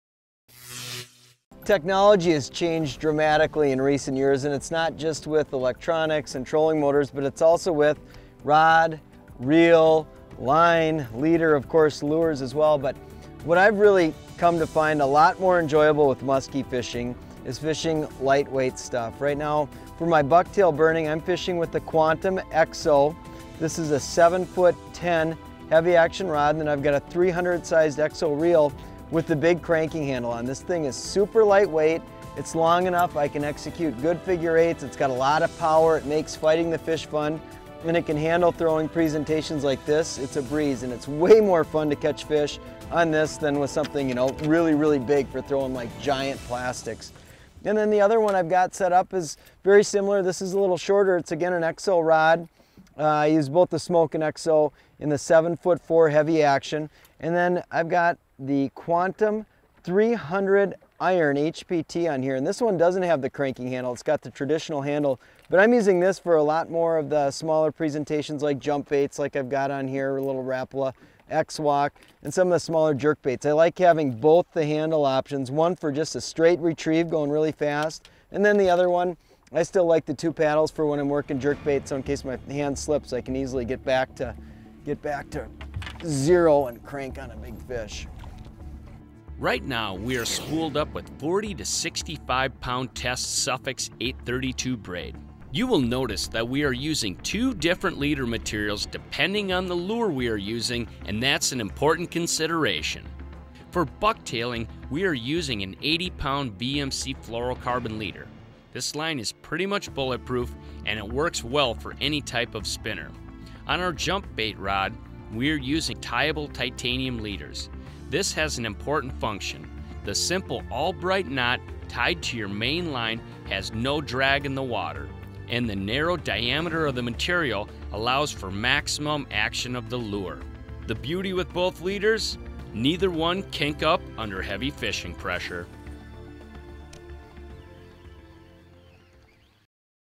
Heavy Bass Tackle for Musky Fishing Fun
Fishing modest-sized lures on heavy bass tackle reduces the physical demands of musky fishing. Anyone can catch fish with properly balanced equipment and tackle.